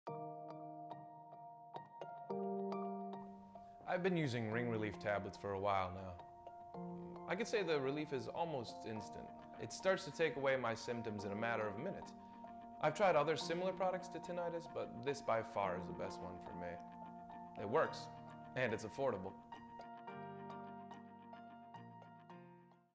The Relief Products™ Ring Relief® Testimonial - Eric helping address tinnitus
A great testimonial from Eric on how Ring Relief® Fast Dissolving Tablets™ have helped his tinnitus symptoms